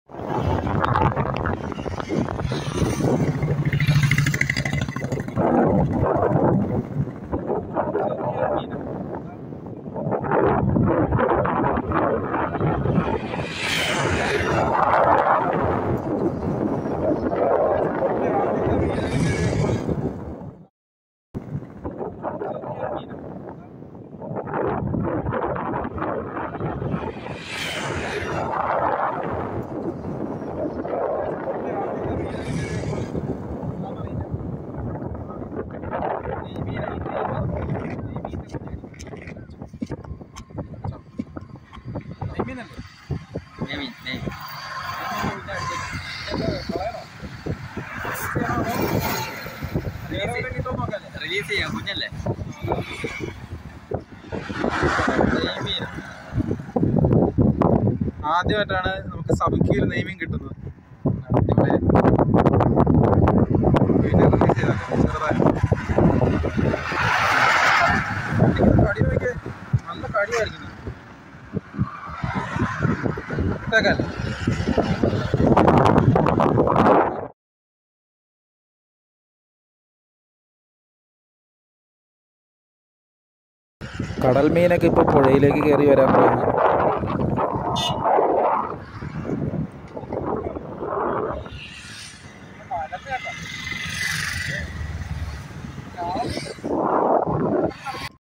... പുഴയിലേക്ക് കയറിവന്ന രാജമത്സ്യം.!.King fish catching on sabiki rig /kerala fishing
സബികി ചൂണ്ടയിൽ അടിച്ച നെയ്മീൻ..
പുഴയിൽ നിന്ന് കിട്ടിയ കടൽ മീൻ..
Sabiki fishing kerala
King fish catching on sabiki rig
Kerala fishing malayalam.